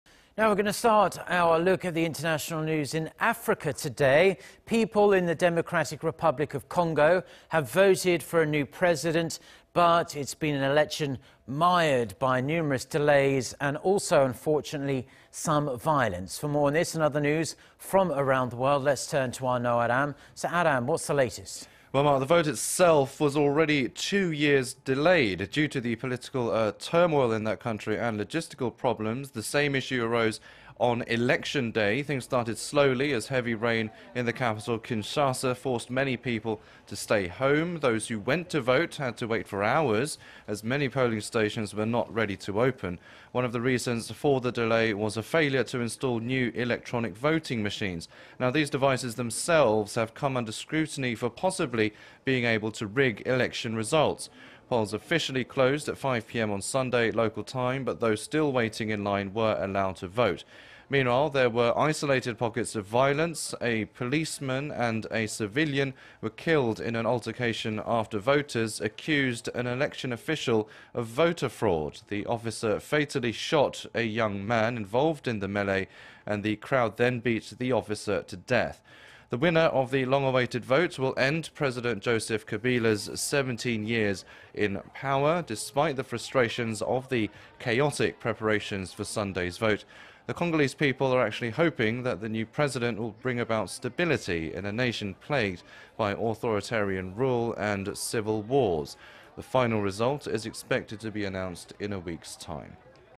DR Congo public vote in tense, long-delayed presidential election
민주콩고, 혼란•폭력사태 속 대선 실시

People in the Democratic Republic of Congo have voted for a new president in an election mired by delays and violence.
For more on this and other news around the world we turn to our Ro Aram…
Aram… what's the latest?

Well Mark… The vote itself was already two years late due to political turmoil and logistical problems.
The same issue arose on Election Day.
Things started slowly as heavy rain in the capital, Kinshasa, forced many people to stay home.
Those who went to vote had to wait for hours as many polling stations were not ready to open.
One of the reasons for the delay was a failure to install new electronic voting machines.
These devices themselves have come under scrutiny for possibly being able to rig elections results.
Polls officially closed at 5 p.m. on Sunday, local time, but those still waiting in line were allowed to vote.
Meanwhile, there were isolated pockets of violence.
A policeman and a civilian were killed in an altercation after voters accused an election official of voter fraud.
The officer fatally shot the young man involved in the melee and the crowd then beat the officer to death.
The winner of the long-awaited vote will end President Joseph Kabila's 17 years in power.
Despite the frustrations of the chaotic preparations for Sunday's vote, the Congolese people are hoping that the new president will bring about stability in a nation plagued by authoritarian rule and civil wars.
The final result is expected to be announced in a week's time.